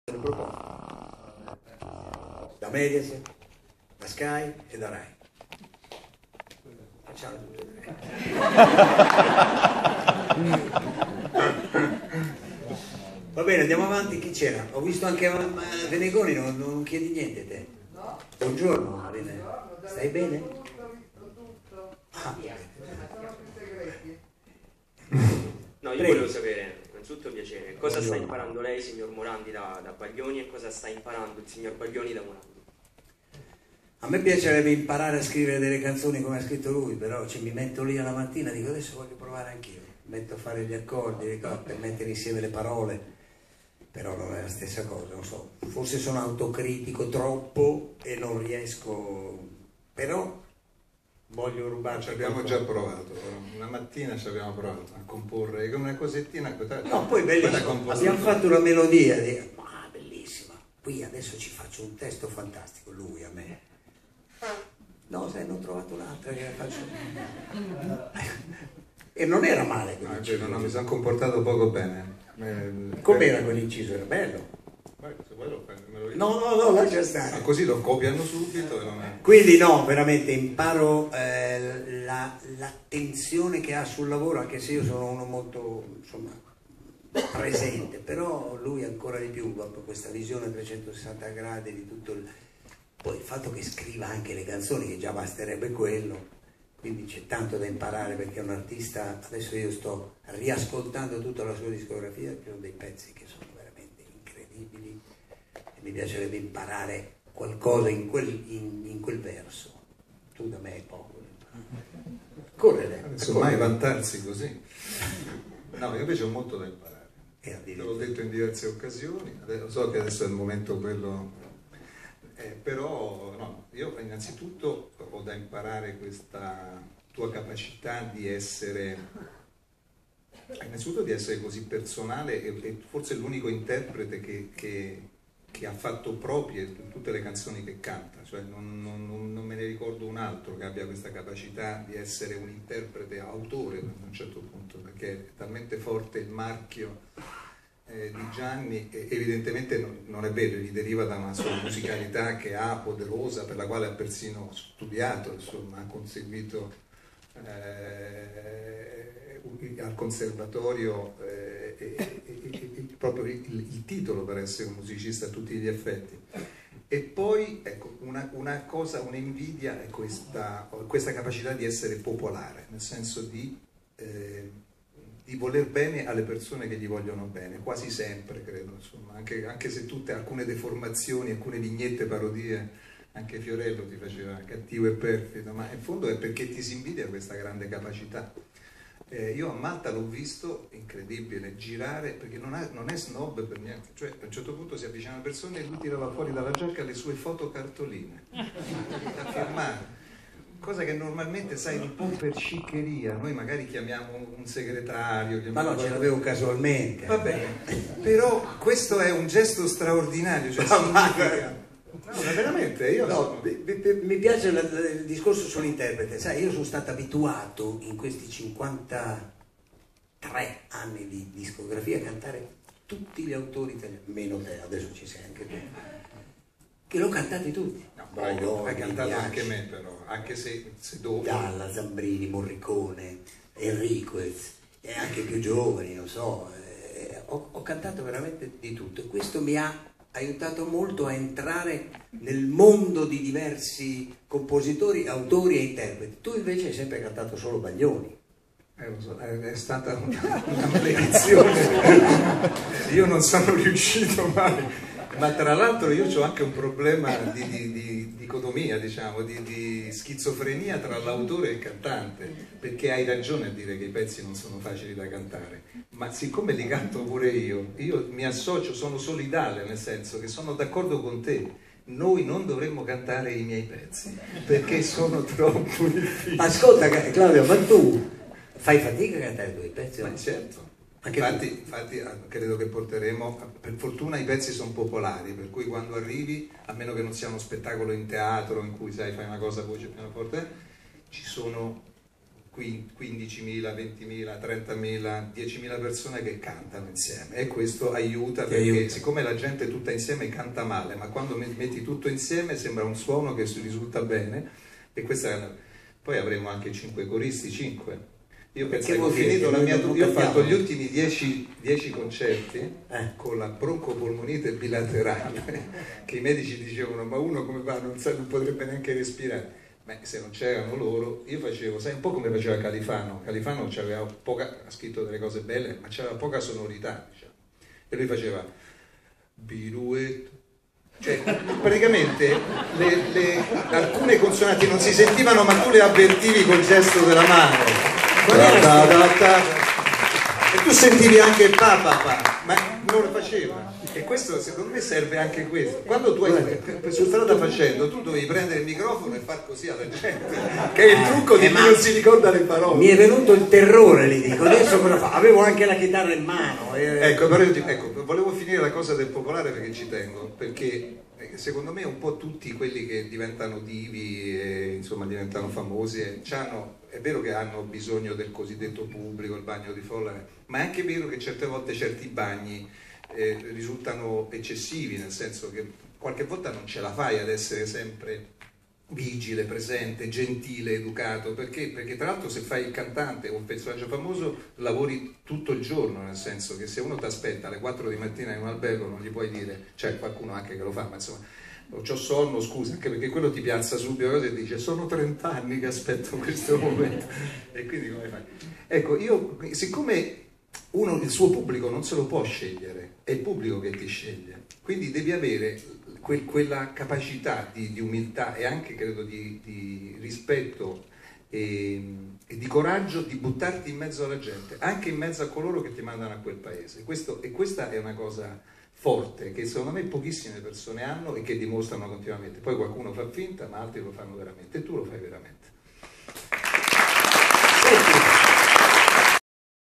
Gianni Morandi e Claudio Baglioni Canzoni Musica Italiana Roma
10 concerti al Foro Italico di Roma per Claudio Baglioni e Gianni Morandi con tante belle canzoni di musica italiana: presentazione alla Casa del Jazz tra aneddoti e musica live.